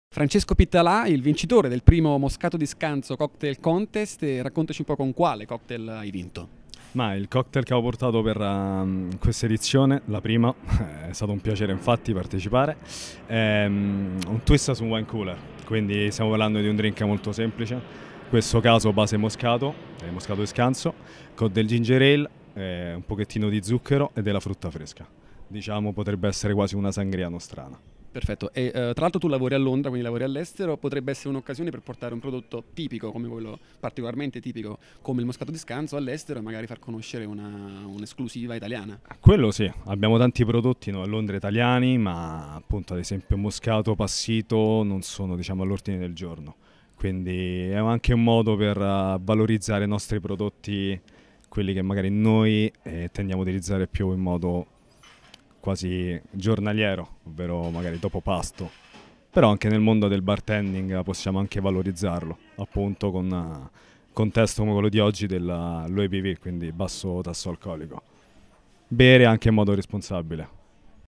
Francesco Pittalà vincitore del primo Moscato di Scanzo Cocktail Contest
Francesco Pittalà del Quaglino's vincitore del primo Moscato di Scanzo Cocktail Contest tenutosi il 17 Settembre presso.